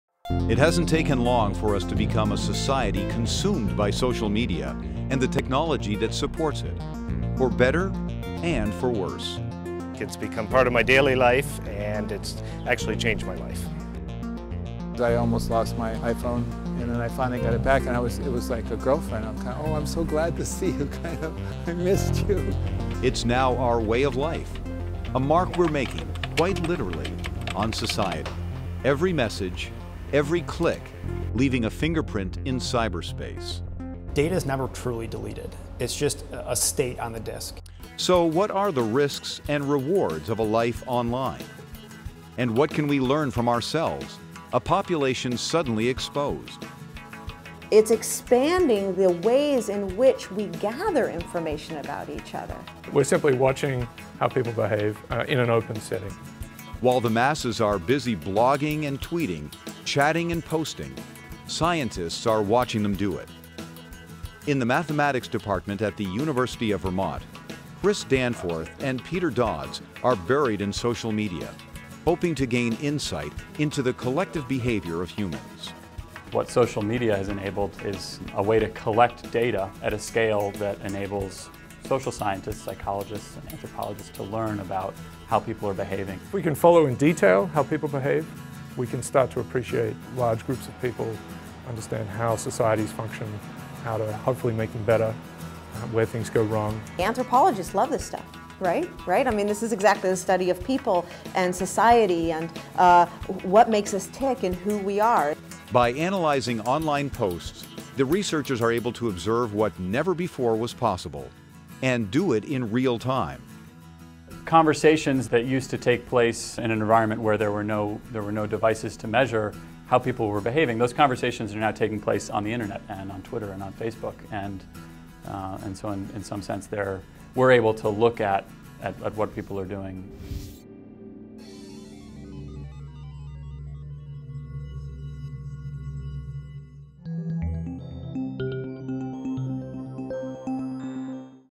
Social Media "Cyber Shadows" Webisode #1
As the popularity of social media grows, so does the interest in mining online data for a variety of uses. Today, our digital "fingerprints" can be traced in ways most of us never imagined -- for market research, criminal investigations and more. A handful of Vermont scientists are busy analyzing our online behavior as a tool for spotting trends and, ultimately, measuring our emotional well-being.